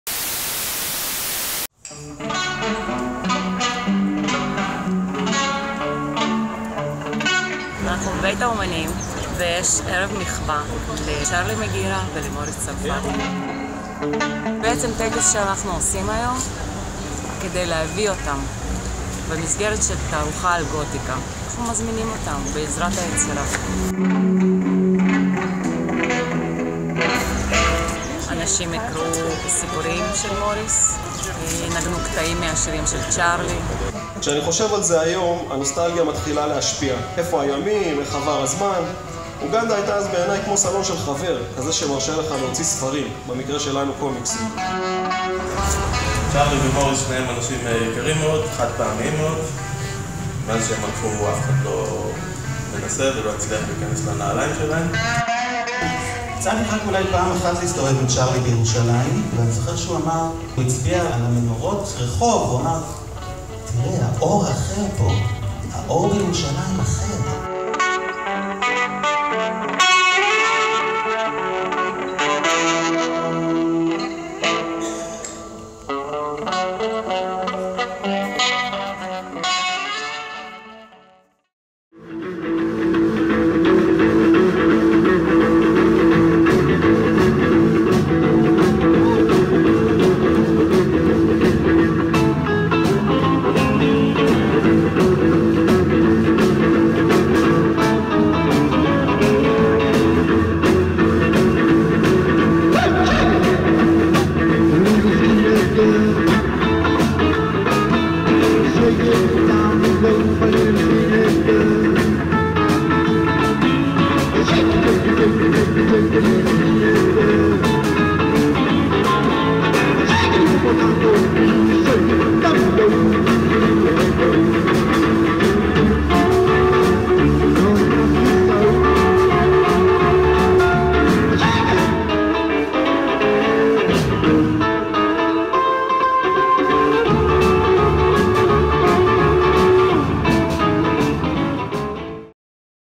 פגישות עם אנשים מיוחדים: ערב לזכרם של צ'רלי מגירה ומוריס צרפתי
כחלק מהתערוכה "אגודת לרווה לחקר העל-טבעי"
צולם בבית האמנים 4.6.2019
צילום אירוע ובימוי: נועם כוזר
עריכה: הילה ספקטור
מוזיקה: צ'רלי מגירה
ביצוע: אדם סבצ'וק אבישי

ההופעה האחרונה של צ'רלי בירושלים
צולם בפרגמון 14.06.16
צילום הופעה: הילה ספקטור

jerusalemite.TV

הופק ע"י
בר-קיימא לתרבות, אמנות, מוסיקה ושלום ע"ר
בתמיכת הקרן לירושלים

ירושלים, יוני 2019